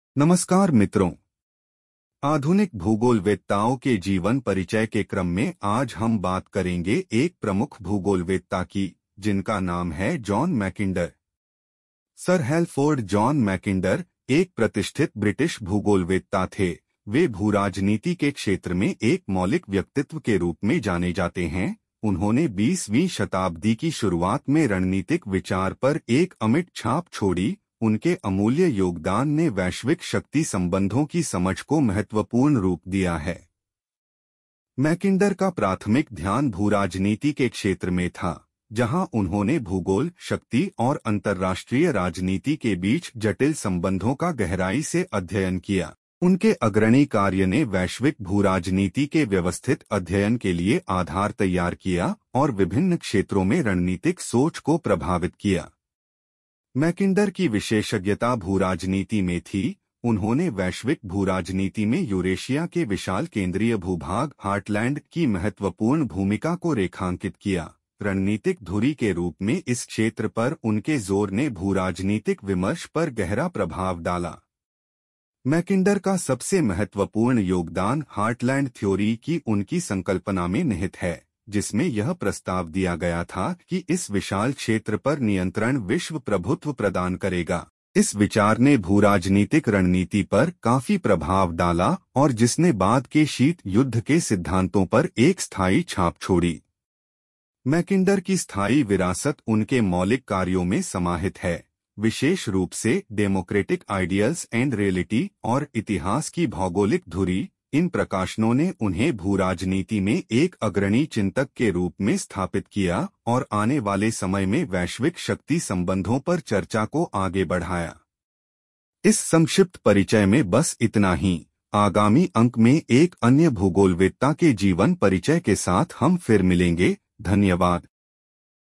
भूगोल और भूगोलवेत्ता जॉन मैकिंडर
भूगोल और भूगोलवेत्ता _ जॉन मैकिंडर (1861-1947)
परिचय:
सर हैलफोर्ड जॉन मैकिंडर, एक प्रतिष्ठित ब्रिटिश भूगोलवेत्ता थे। वे भू-राजनीति के क्षेत्र में एक मौलिक व्यक्तित्व के रूप में जाने जाते हैं। उन्होंने 20 वीं शताब्दी की शुरुआत में रणनीतिक विचार पर एक अमिट छाप छोड़ी। उनके अमूल्य योगदान ने वैश्विक शक्ति संबंधों की समझ को महत्वपूर्ण रूप दिया है।
कार्य का क्षेत्र
मैकिंडर का प्राथमिक ध्यान भूराजनीति में के क्षेत्र था, जहां उन्होंने भूगोल, शक्ति और अंतर्राष्ट्रीय राजनीति के बीच जटिल संबंधों का गहराई से अध्ययन किया। उनके अग्रणी कार्य ने वैश्विक भू-राजनीति के व्यवस्थित अध्ययन के लिए आधार तैयार किया और विभिन्न क्षेत्रों में रणनीतिक सोच को प्रभावित किया।
विशेषज्ञता
मैकिंडर की विशेषज्ञता भूराजनीति में थी। उन्होंने वैश्विक भूराजनीति में यूरेशिया के विशाल केंद्रीय भूभाग "हार्टलैंड" की महत्वपूर्ण भूमिका को रेखांकित किया। रणनीतिक धुरी के रूप में इस क्षेत्र पर उनके जोर ने भू-राजनीतिक विमर्श पर गहरा प्रभाव डाला।
महत्वपूर्ण योगदान
मैकिंडर का सबसे महत्वपूर्ण योगदान "हार्टलैंड थ्योरी" की उनकी संकल्पना में निहित है, जिसमें यह प्रस्ताव दिया गया था कि इस विशाल क्षेत्र पर नियंत्रण विश्व प्रभुत्व प्रदान करेगा। इस विचार ने भूराजनीतिक रणनीति पर काफी प्रभाव डाला और जिसने बाद के शीत युद्ध के सिद्धांतों पर एक स्थायी छाप छोड़ी।
प्रमुख कृतियाँ
मैकिंडर की स्थायी विरासत उनके मौलिक कार्यों में समाहित है, विशेष रूप से "डेमोक्रेटिक आइडियल्स एंड रियलिटी" (1919) और "इतिहास की भौगोलिक धुरी" (1904)। इन प्रकाशनों ने उन्हें भूराजनीति में एक अग्रणी चिंतक के रूप में स्थापित किया और आने वाले समय में वैश्विक शक्ति संबंधों पर चर्चा को आगे बढ़ाया।